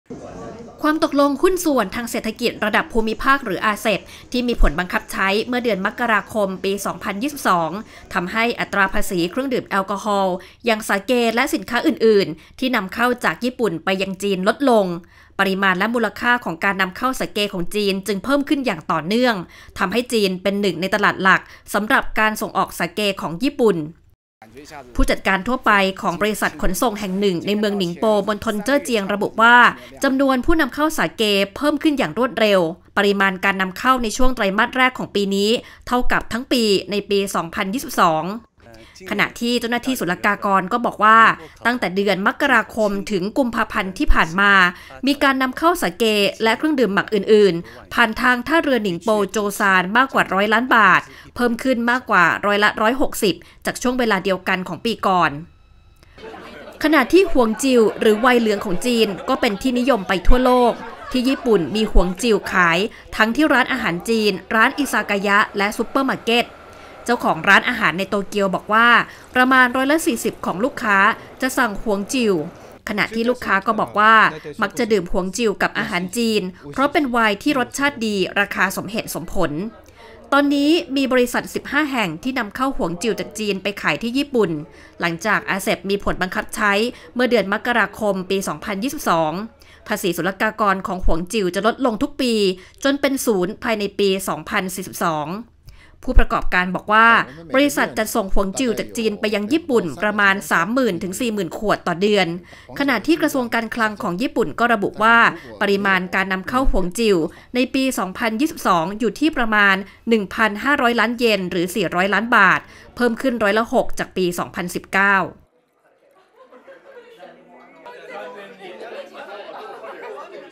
ความตกลงหุ้นส่วนทางเศรษฐกิจระดับภูมิภาค (RCEP) กระตุ้นการส่งออกเครื่องดื่มแอลกอฮอล์ (พากย์ไทย)
ความตกลงหุ้นส่วนทางเศรษฐกิจระดับภูมิภาค (RCEP) ช่วยกระตุ้นการส่งออกเครื่องดื่มแอลกอฮอล์ระหว่างจีนและญี่ปุ่น (พากย์ไทย)
ความตกลงหุ้นส่วนทางเศรษฐกิจระดับภูมิภาค (RCEP) ที่มีผลบังคับใช้เมื่อเดือนมกราคม ค.ศ. 2022 ทำให้อัตราภาษีเครื่องดื่มแอลกอฮอล์ อย่างสาเกและสินค้าอื่น ๆ ที่นําเข้าจากญี่ปุ่นไปยังจีนลดลง ปริมาณและมูลค่าของการนําเข้าสาเกของจีนเพิ่มขึ้นอย่างต่อเนื่องในปีที่ผ่านมา ทําให้จีนเป็นหนึ่งในตลาดหลักสําหรับการส่งออกสาเกของญี่ปุ่น
Li Sun ผู้จัดการทั่วไปของบริษัทโลจิสติกส์แห่งหนึ่งในเมืองหนิงโป มณฑลเจ้อเจียง ระบุว่า จํานวนผู้นําเข้าสาเกเพิ่มขึ้นอย่างรวดเร็วตั้งแต่ปีค.ศ. 2021 ปริมาณการนําเข้าในช่วงไตรมาสแรกของปีนี้เท่ากับทั้งปีในปีค.ศ.2022 ขณะที่ Wang Guohua เจ้าหน้าที่ศุลกากร กล่าวว่า ตั้งแต่เดือนมกราคมถึงกุมภาพันธ์ปีนี้ มีการนำเข้าสาเกและเครื่องดื่มหมักอื่นๆ ผ่านทางท่าเรือหนิงโป-โจวซานมากกว่า 100 ล้านบาท เพิ่มขึ้นมากกว่าร้อยละ 160 จากช่วงเวลาเดียวกันของปีก่อน
“หวงจิ่ว (Huangjiu)” หรือไวน์เหลืองของจีน เป็นที่นิยมไปทั่วโลก ที่ญี่ปุ่นมี “หวงจิ่ว” ขายทั้งที่ร้านอาหารจีน ร้านอิซากายะ และซูเปอร์มาร์เก็ต เจ้าของร้านอาหารในโตเกียวบอกว่า ประมาณร้อยละ 40 ของลูกค้าจะสั่ง หวงจิ่วขณะที่ลูกค้าบอกว่า มักจะดื่มหวงจิ่วกับอาหารจีน เพราะเป็นไวน์ที่รสชาติดี ราคาสมเหตุสมผล
ตอนนี้มีบริษัท 15 แห่งที่นำเข้าหวงจิ่วจากจีนไปขายที่ญี่ปุ่น หลังจาก RCEP มีผลบังคับใช้เมื่อเดือนมกราคมค.ศ. 2022 ภาษีศุลกากรของหวงจิ่วจะลดลงทุกปี จนเป็นศูนย์ภายในปีค.ศ. 2042
Guo Bin ผู้ประกอบการกล่าวว่า บริษัทจัดส่งหวงจิ่วจากจีนไปยังญี่ปุ่น ประมาณ 30,000-40,000 ขวดต่อเดือน  ขณะที่กระทรวงการคลังของญี่ปุ่น ระบุว่า ปริมาณการนําเข้าหวงจิ่วในปีค.ศ. 2022 อยู่ที่ประมาณ 1,500 ล้านเยน หรือ 400 ล้านบาท เพิ่มขึ้นประมาณร้อยละ 6 จากปีค.ศ. 2019